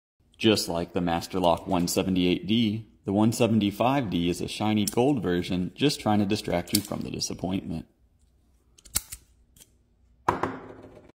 Master Lock 175D Bypass - Lock Picking
Bypassing a Master Lock 175D combination lock